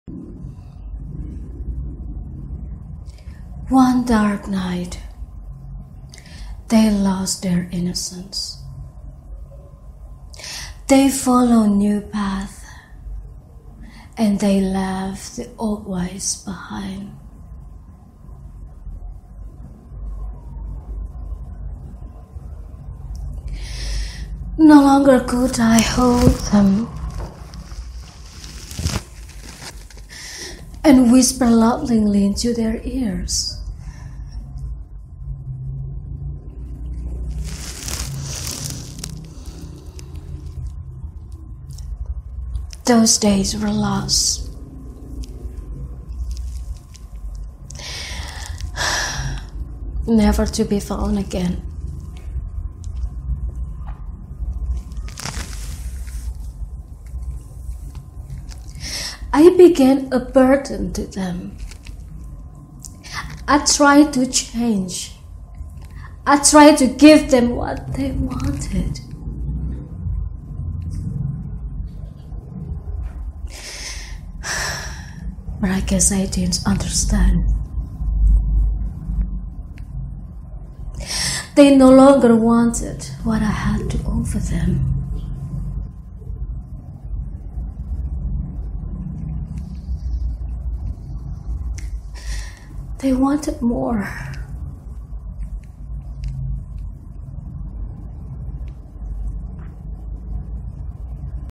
Mother Earth Part 3 monologue for actors written by D. M. Larson
This is part 3 of the monologue "Mother Earth" for a female (woman) actress for performance, competition, workshop or classroom use for acting.